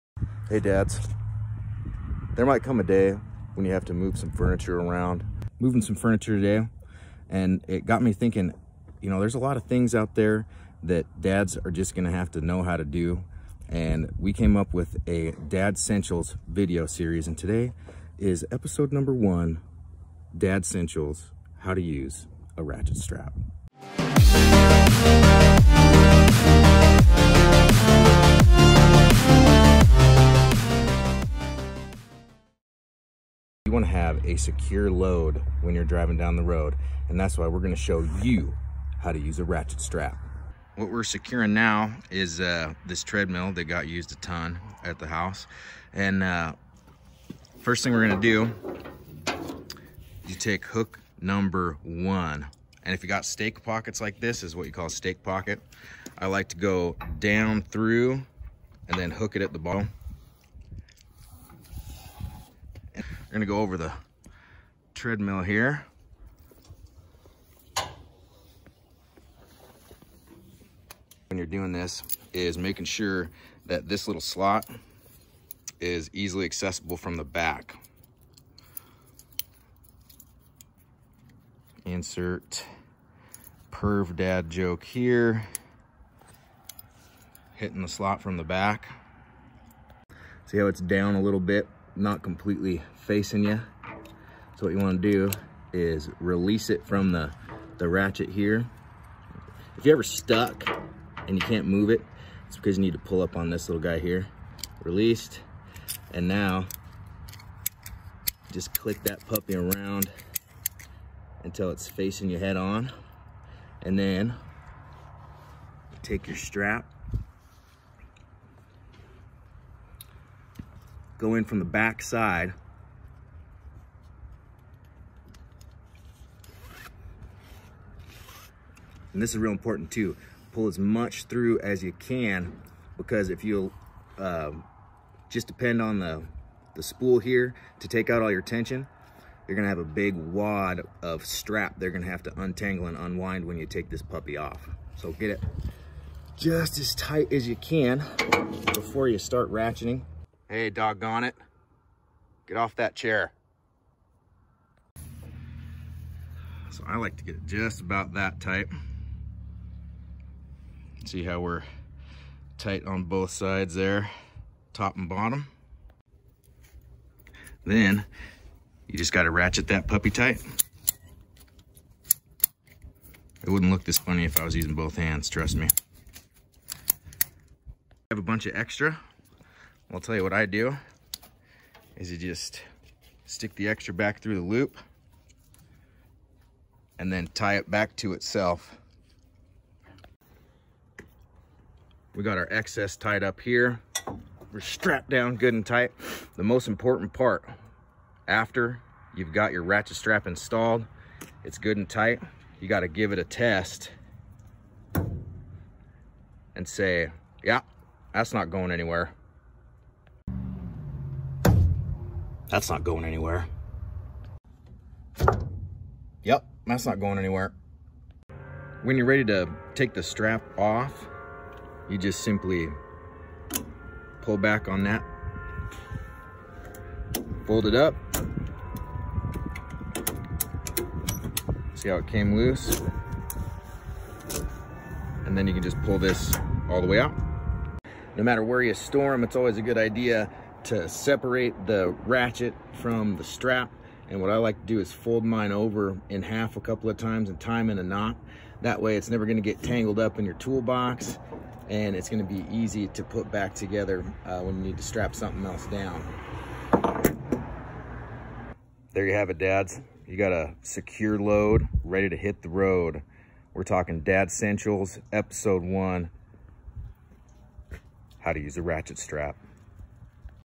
Dad-Sentials Ep 1: How To Use A Ratchet Strap
In this Dad-Sential video, Sam will explain and show you an essential tip that every dad needs to know. How to use a ratchet strap. Take notes, this will come in handy when you're doing your dump runs, securing loads, and just playing with the ratchet.